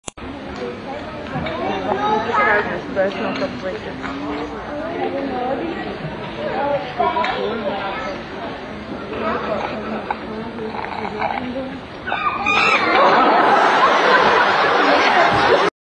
Did it ever happen to you?
Monkey seeing its face in mirror and got shocked because of its beauty?  Did it ever happen to you?